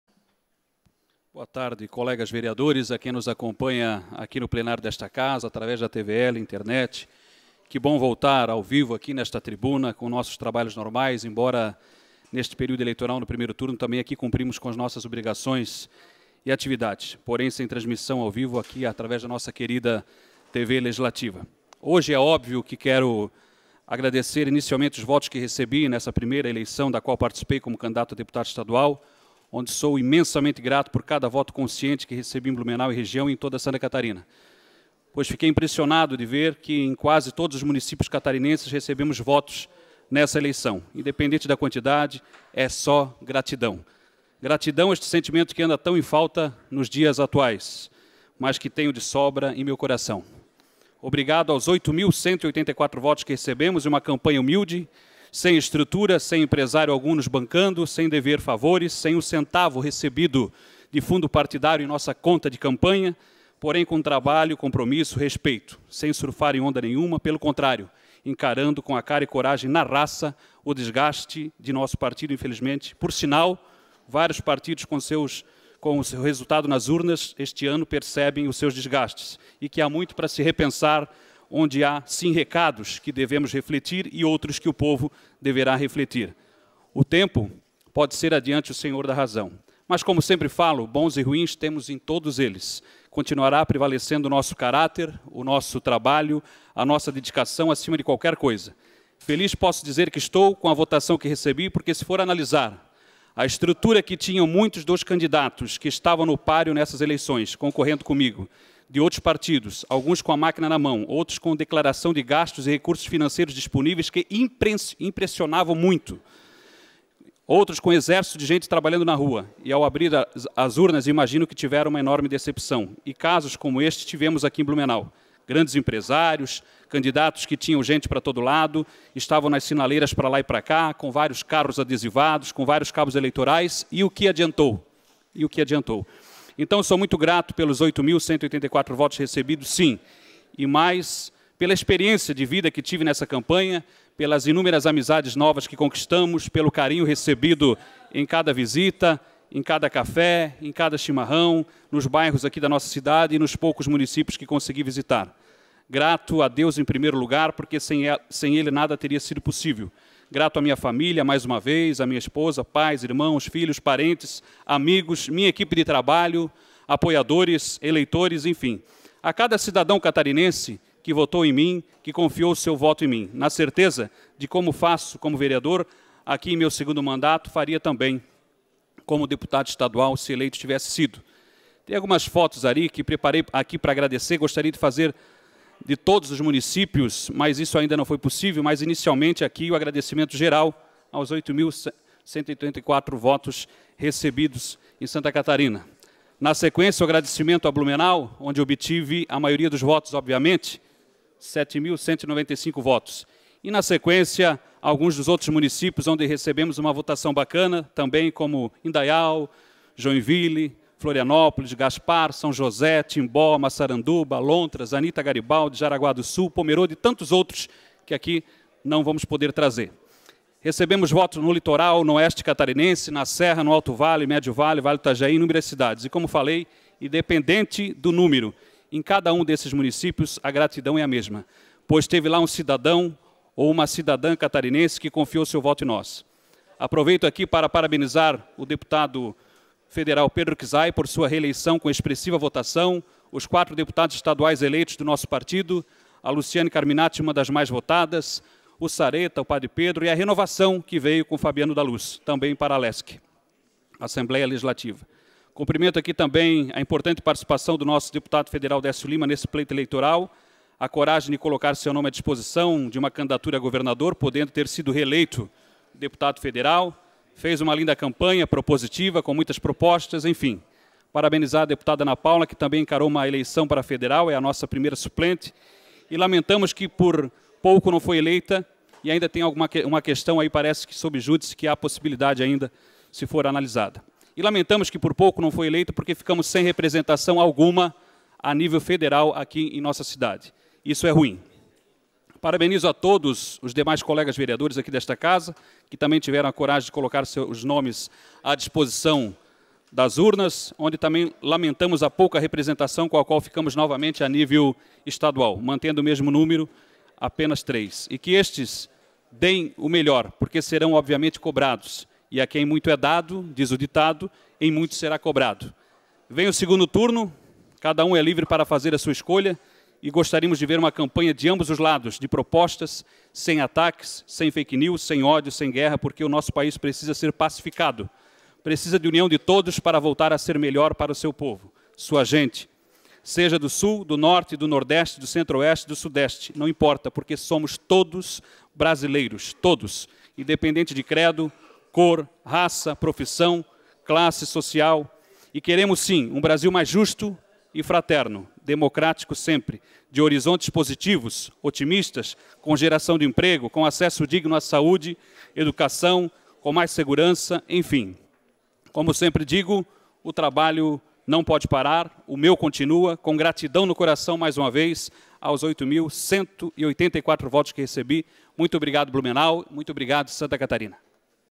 Pronunciamento Vereador Adriano Pereira - 09/10/2018
Vereador Adriano Pereira agradece eleitores e destaca que sua campanha foi feita com a colaboração da equipe, família e amigos
O vereador Adriano Pereira (PT) agradeceu todos os votos que recebeu dos eleitores catarinenses. Disse que obteve votos em muitos municípios do Médio e Alto Vale, do Litoral, da Região Norte e Oeste. Ressaltou que fez a sua campanha sem estrutura, sem apoio de empresários, sem fundo partidário, assinalando que na comparação com outros candidatos que tinham uma grande estrutura e a máquina a seu dispor, esses tiveram uma grande decepção. Destacou que apesar de não ter conquistado a vitória, a campanha lhe trouxe uma grande experiência e novas amizades. Agradeceu especialmente aos eleitores de Blumenau, que deram a ele 7.905 votos dos 8.134 recebidos no total. Parabenizou os candidatos petistas eleitos e destacou a deputada estadual Ana Paula Lima, que concorria a uma vaga a deputada federal, não se elegeu, e isso fez com que Blumenau ficasse sem nenhuma representação na Câmara. Também parabenizou a campanha do deputado Décio Lima a governador. Agradeceu a Deus, à família, aos apoiadores e a sua equipe, por terem sido todos incansáveis. Enfatizou a confiança que as 8.134 pessoas por todo o Estado depositaram nele. Ressaltou que o trabalho comprometido e sério vai continuar.